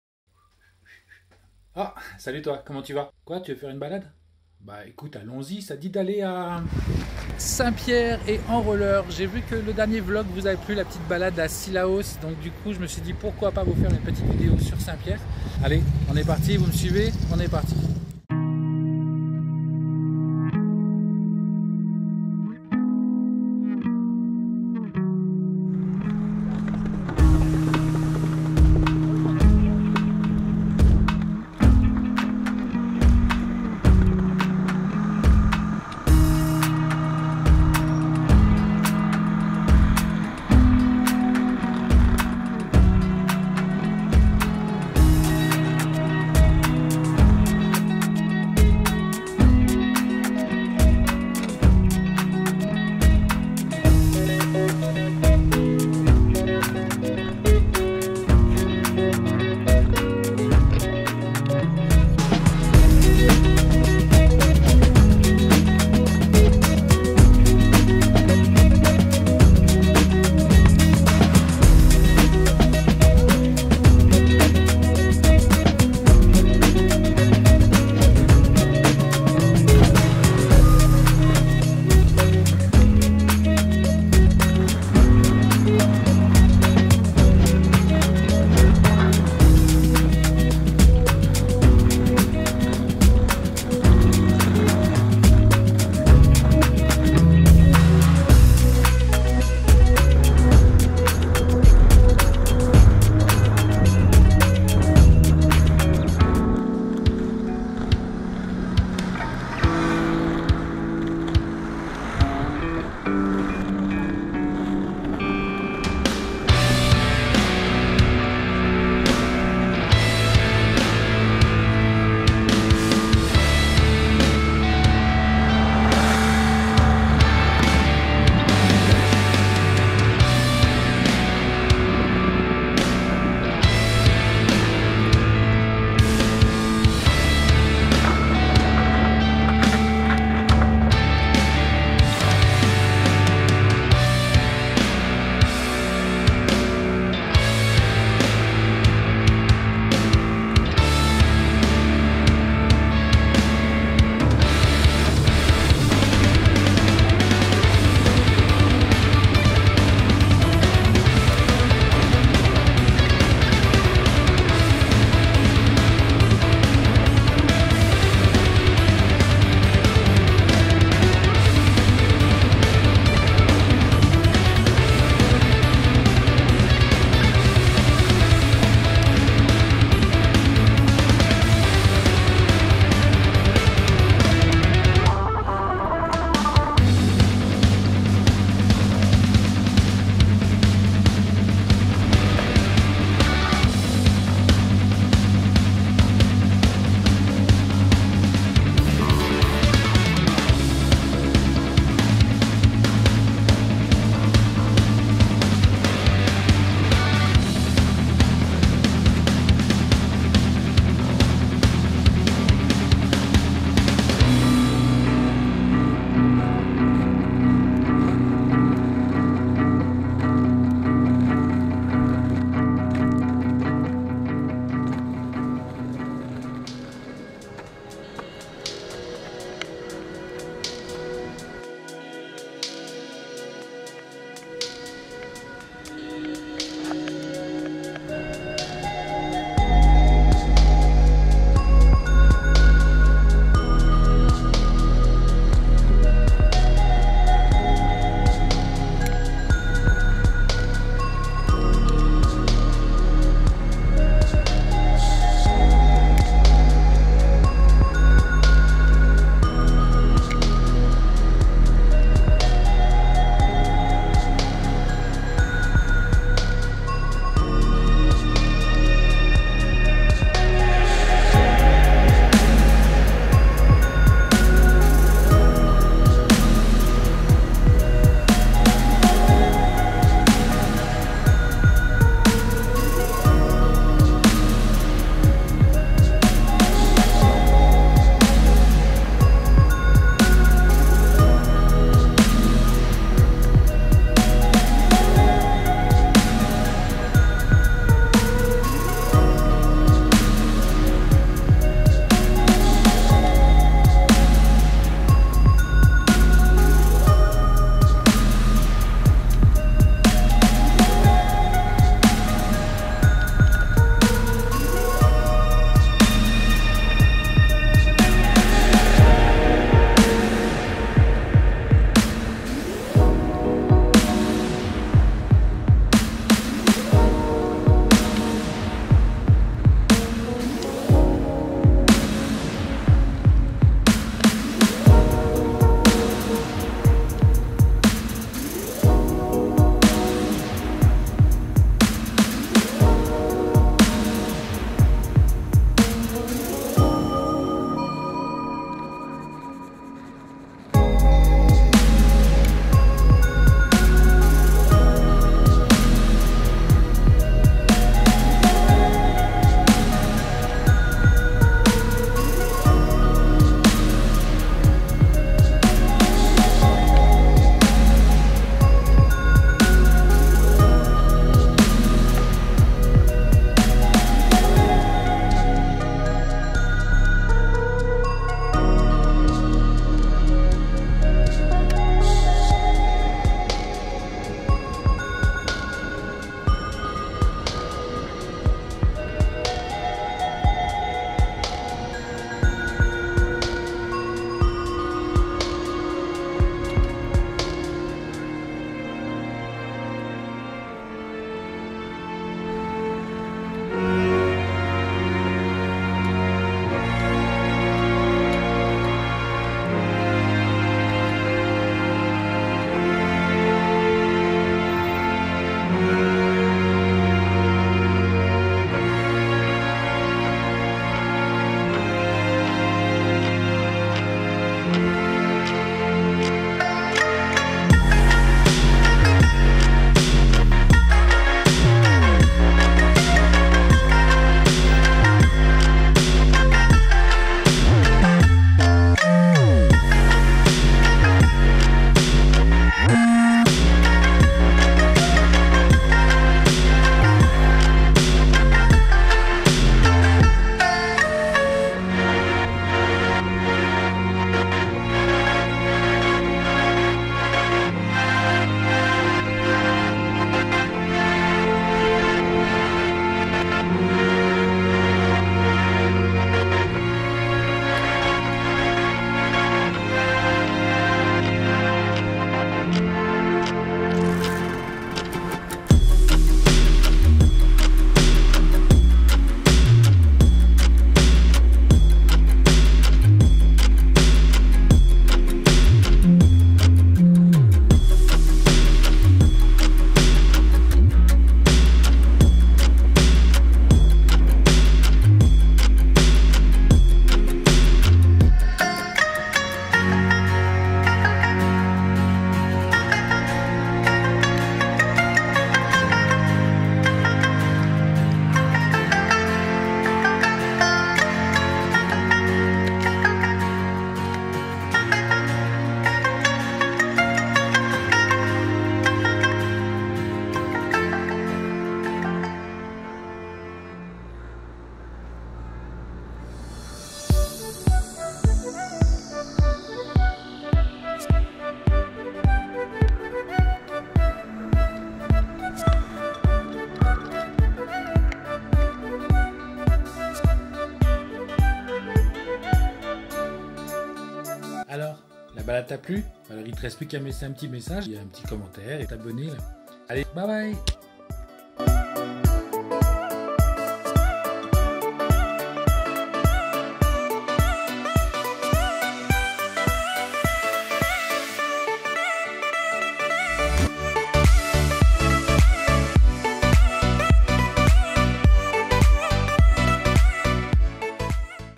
BALADE EN ROLLER A ST PIERRE ILE DE LA REUNION
je te fais visiter St Pierre sur l'île de la Réunion , au travers d'une balade à roller.

Commente la vidéo et partage la